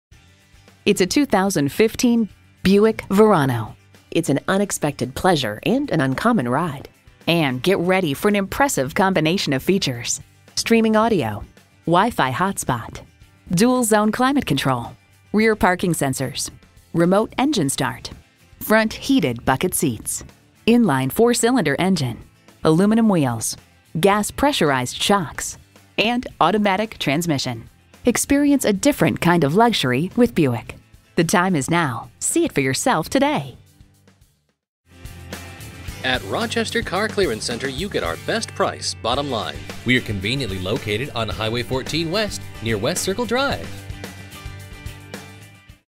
2015 Buick Verano Rochester Winona, MN KB20532 - SOLD
Year: 2015
Make: Buick
Model: Verano
Trim: Convenience Group
Engine: ECOTEC 2.4L I4 SIDI DOHC VVT
Transmission: 6-Speed Automatic Electronic with Overdrive
Color: Brown
Interior: Cashmere
Mileage: 32501
Stock #: KB20532
VIN: 1G4PR5SK5F4198488
Recent Arrival! Verano Convenience Group Mocha Bronze Metallic Odometer is 30005 miles below market average! CARFAX One-Owner. Clean CARFAX. We look forward to earning your business!

Tune-Ups
Transmission Service
Brake Service
Performance Parts
Factory Wheels

Address:
2980 HWY 14